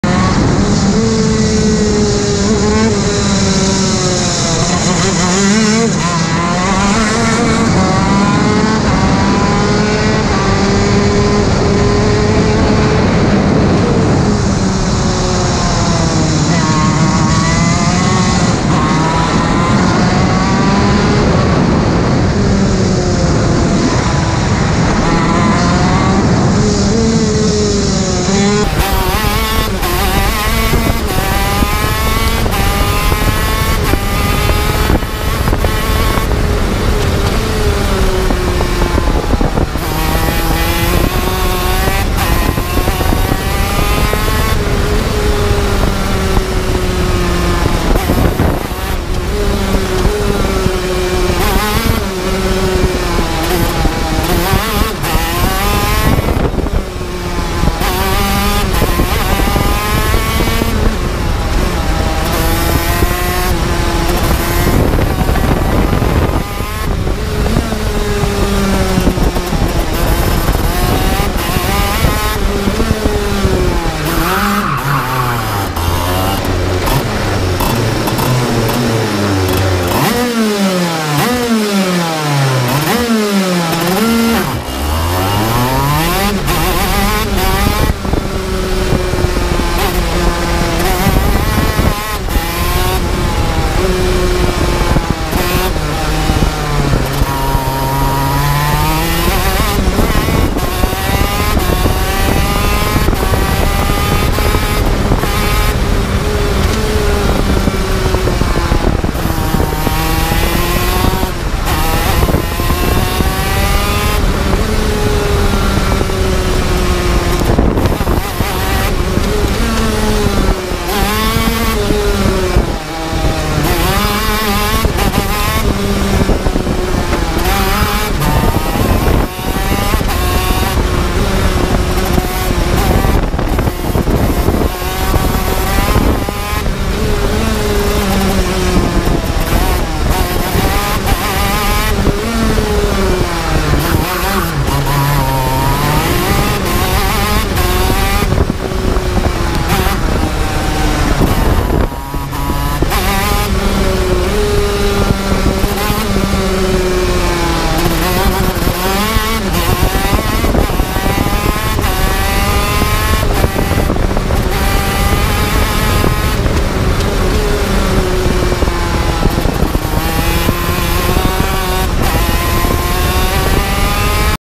KARTING ll INTREPID KZ TM K9C ll SERRES CIRCUIT
- Second time driving ever my intrepid KZ K9C
Driver : John Moumos
Chassis : Intrepid Cruiser
Track : Serres circuit
Cameras : Go pro Hero 4 and 5  black